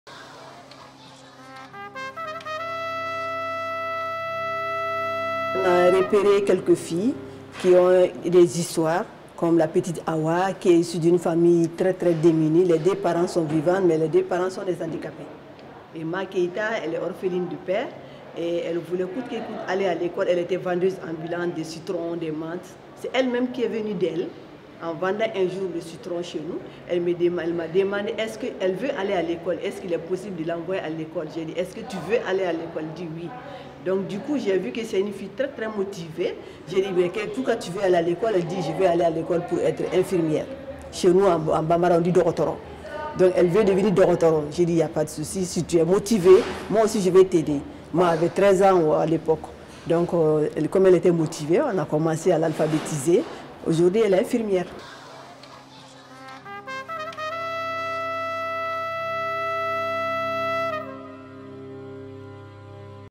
Ensemble, c'est possible ! - Réaction de Mariam Sidibé
Réaction de Mariam Sidibé, responsable du centre éducatif au Mali, suite à la projection de l'Avant-Première du film "Ensemble, c'est possible !" le 25 novembre 2016. Grâce à elle, des filles ont pu réaliser leur projet : devenir infirmière.

Tourné en 2016 dans deux centres éducatifs au Mali et au Liban, ce court-métrage de 28 minutes réalisé par Safy Nebbou invite à retrouver confiance dans l'humain.
Un support sensible pour débattre de la solidarité, du handicap, de l'éducation et de notre capacité à vivre ensemble.
Un film pour redonner l'élan et retrouver l'envie d'agir !

Voix off : Charles Berling